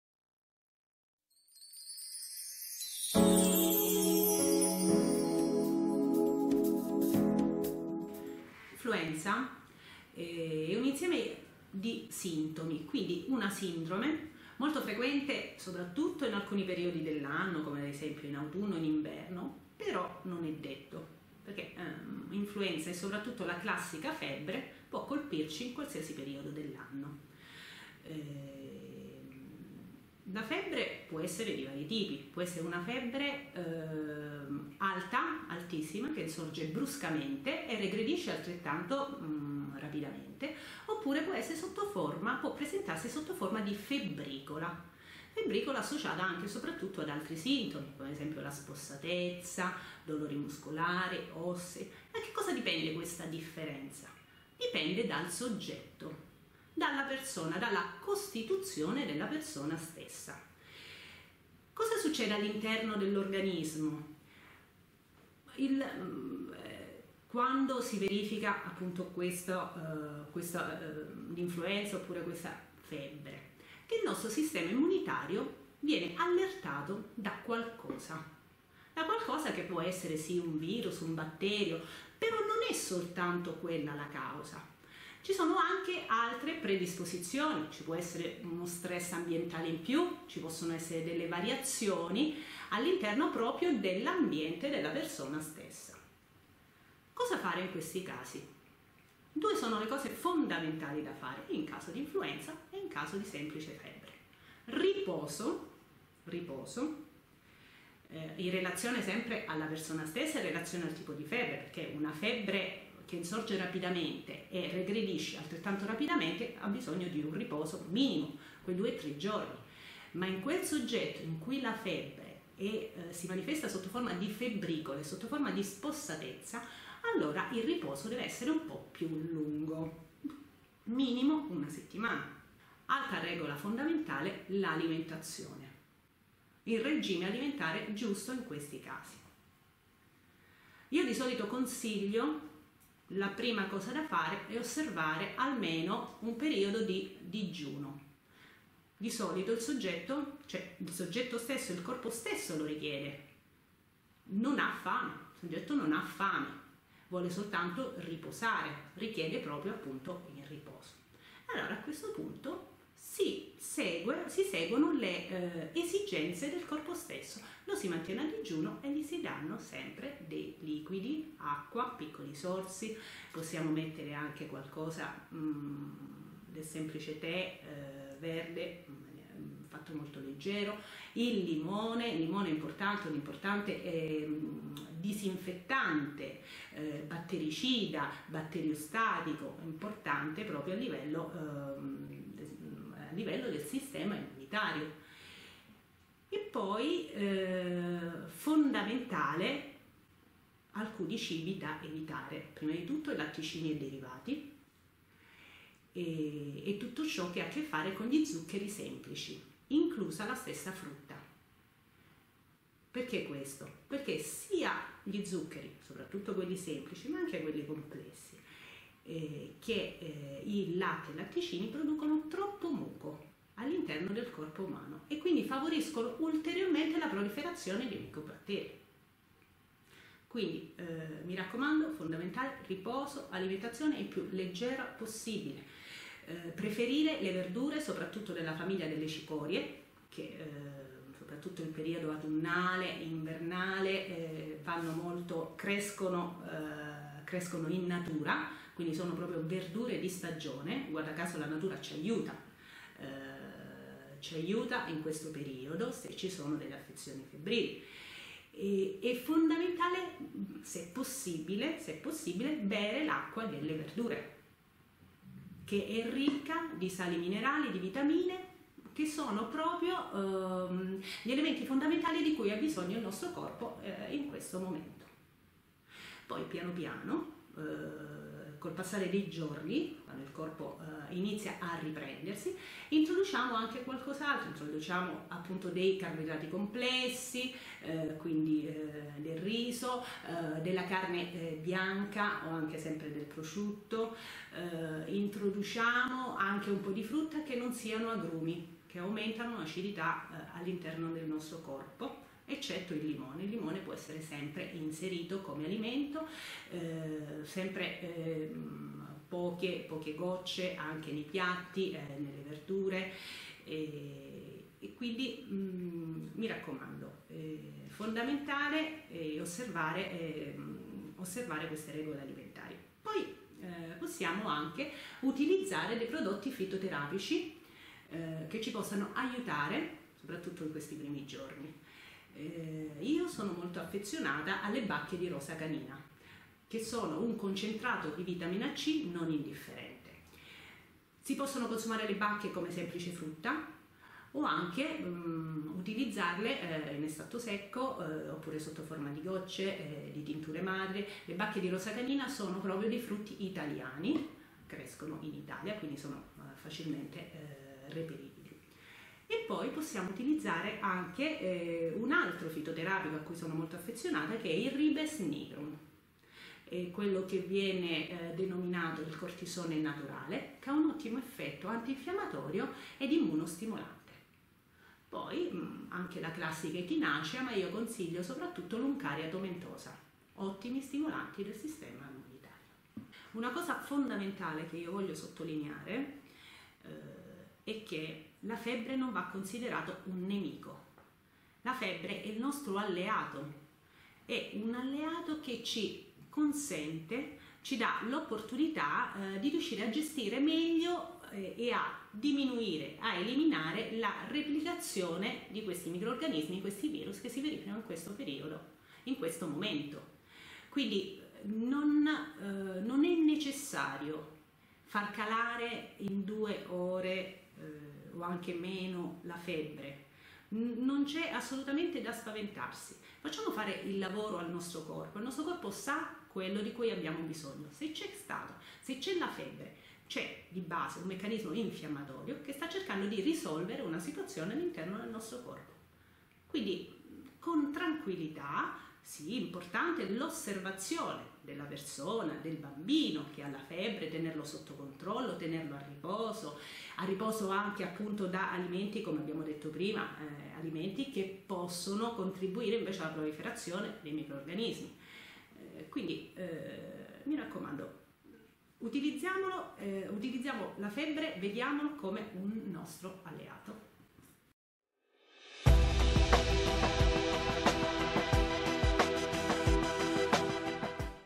Febbre e influenza: cosa mangiare e rimedi naturali efficaci
Parliamo di febbre e influenza. Come dobbiamo comportarci e cosa è meglio mangiare? La dottoressa Buccarella ci dà alcuni consigli utili e ci suggerisce degli efficaci rimedi naturali da sperimentare.

Paola Assunta Buccarella è un medico internista, omeopata e nutrizionista. Vive e lavora a Roma ed autrice del libro "Dimagrire, che stress!"